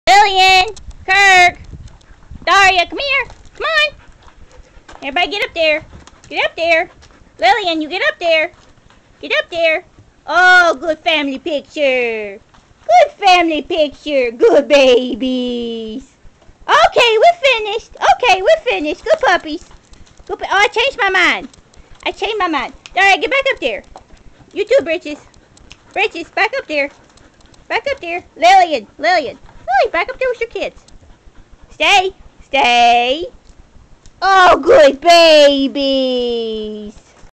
Magic Hour Poodles posing for pictures!
Lillian and her two 10 month pups, Kirk and Daria, are kind enough to listen to my request to go to one spot for photos.  =D